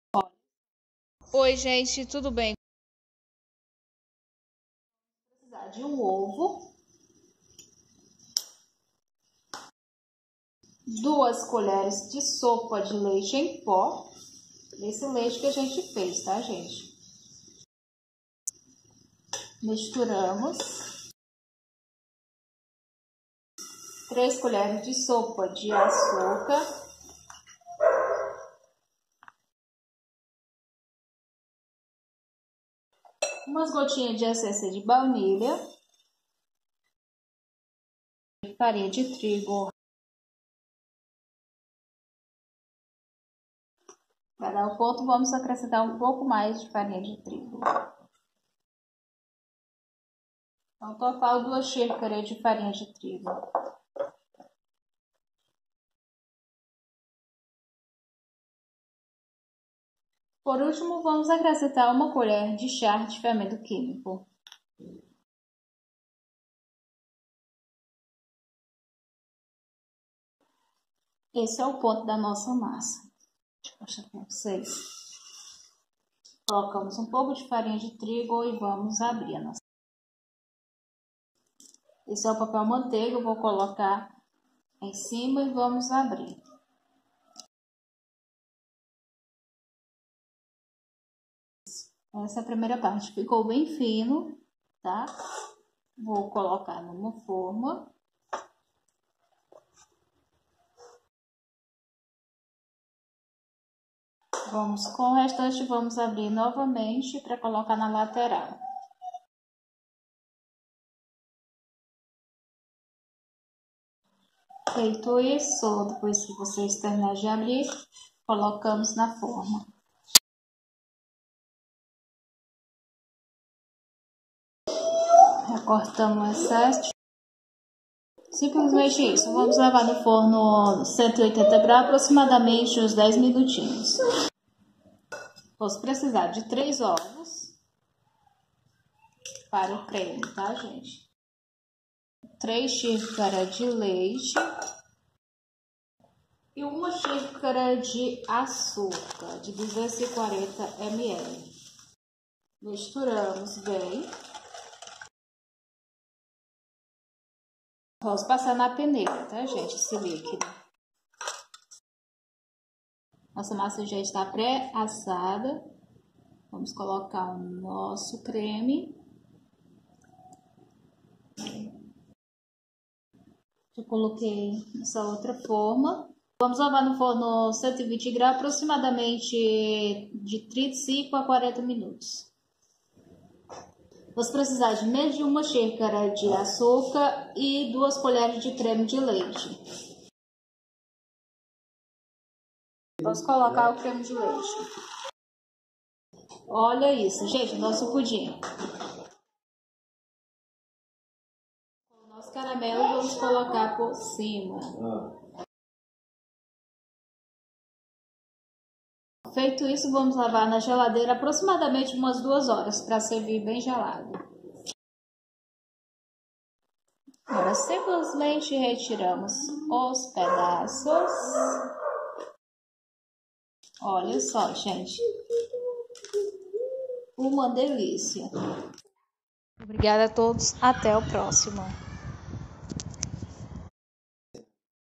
torta pudim sem leite condensado sem creme de leite/super fácil econômico😋👌
Todos bem-vindo!

Ingredientes
 para a torta vamos precisar 💁‍♀️
1 ovo
2 colheres de sopa de leite em pó
3 colheres de sopa de açúcar
 umas gotinhas de essência de baunilha]
 2 xícara de farinha de trigo
1 colher de chá de fermento químico.

para o pudim vamos precisar de
3 ovos
3 xícara de leite líquido  (240 ml)
1 xícara de Açúcar de (240 ml)

para o caramelo
A metade de uma xícara de 240 ml de açúcar
2 colheres de sopa de creme de leite😋👌

💁‍♀️o creme de leite apenas no caramelo